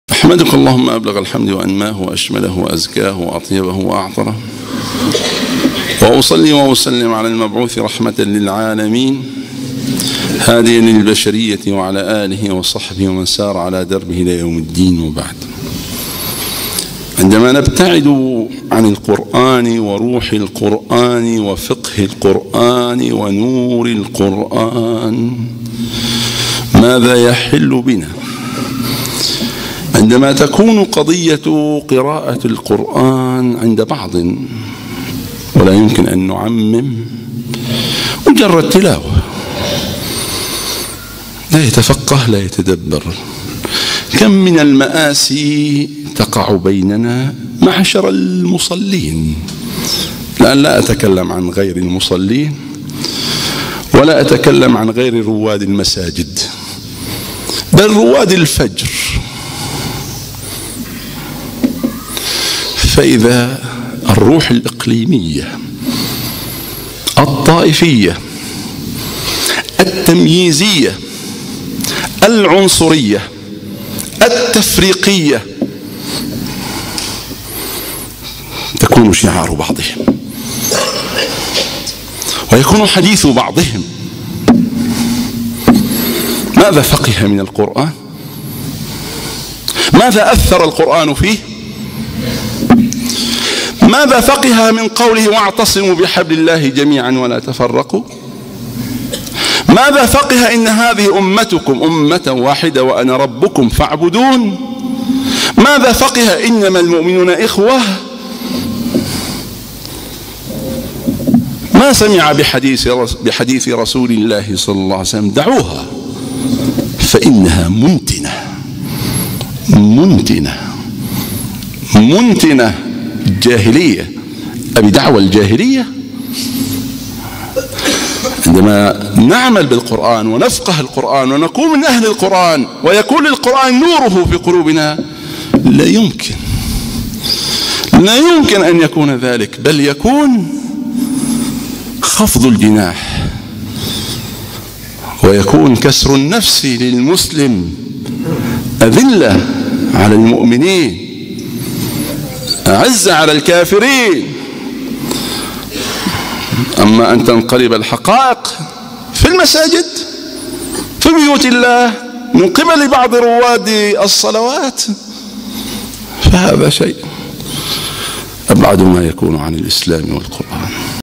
دعوها فإنها منتنة - د. محمد سعيد حوى
الجزء الثاني من خطبة الجمعة للشيخ الدكتور محمد سعيد حوى "دعوهافإنها منتنة" في مسجد السيدة عائشة، الأردن، عمان، عبدون، وللمتابعة في تويتر